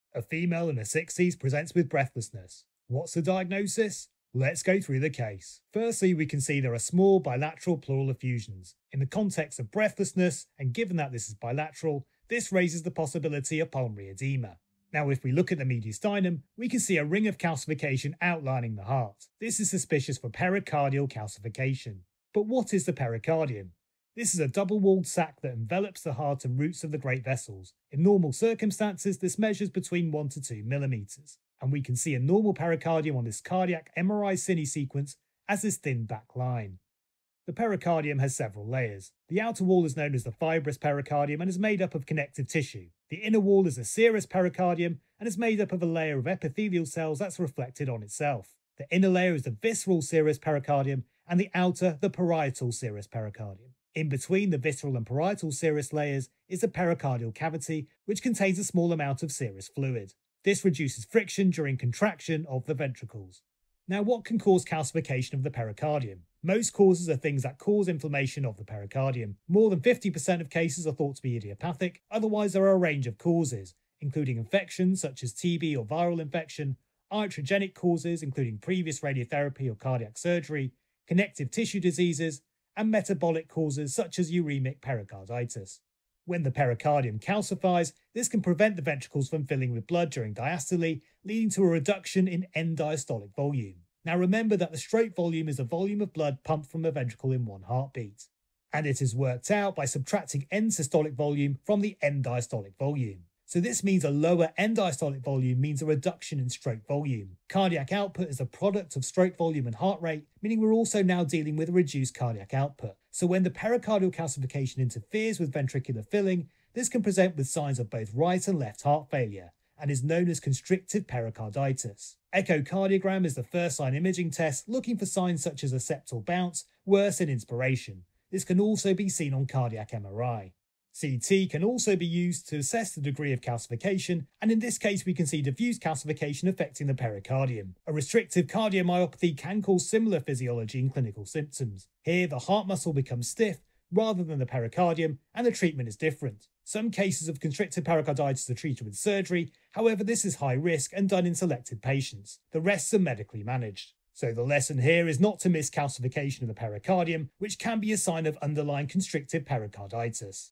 Chest X-Ray breakdown: assessing pericardial calcification
A female in her 60s presents with breathlessness. What does the Chest X-Ray show?

CONSTRICTIVE PERICARDITIS

👨🏽‍💻The first thing to see is that there are bilateral small pleural effusions which in the context of breathlessness raises suspicion of pulmonary oedema

👨🏽‍💻There is also a ring of calcification outlining the heart raising suspicion of pericardial calcification. Other causes of cardiac calcification we can see on X-Ray include a calcified LV aneurysm, which could be a cause here, as well as aortic valve calcification and mitral annulus calcification

👨🏽‍💻 In constrictive pericarditis the pericardium becomes thickened and stiff. This causes the pericardium to restrict the normal expansion and contraction of the heart during the cardiac cycle. As a result, there is a disconnect between the pressures inside the chest and those inside the heart

👨🏽‍💻 During normal respiration, the pressure inside the chest decreases during inhalation, causing blood to flow from the veins into the right atrium and from the lungs into the left atrium. In turn, this increases the volume of blood in the right and left ventricles, allowing the heart to pump more blood to the body

👨🏽‍💻 However, in constrictive pericarditis, the thickened and stiff pericardium limits the expansion of the heart, preventing it from filling with blood during inhalation. This results in a reduced diastolic filling of the left ventricle. Moreover, the pressure changes inside the chest are not fully transmitted to the left atrium and ventricle, resulting in a dissociation between the intrathoracic and intracardiac pressures

👨🏽‍💻 This creates a situation where the pressure gradient between the pulmonary capillary wedge pressure and left ventricular diastolic pressure remains constant during the respiratory cycle

👨🏽‍💻 To compensate for the reduced filling of the left ventricle, the right ventricle fills with more blood during inhalation. The increased blood flow to the right atrium during inhalation is mainly from the inferior vena cava, as it is not affected by the changes in intrathoracic pressure. Additionally, the increased transabdominal pressure during inhalation assists in pushing blood from the inferior vena cava into the right atrium

👨🏽‍💻 This leads to an increased jugular venous pressure during inhalation, which is known as Kussmaul's sign. During exhalation, the opposite changes occur in the filling of the left and right sides of the heart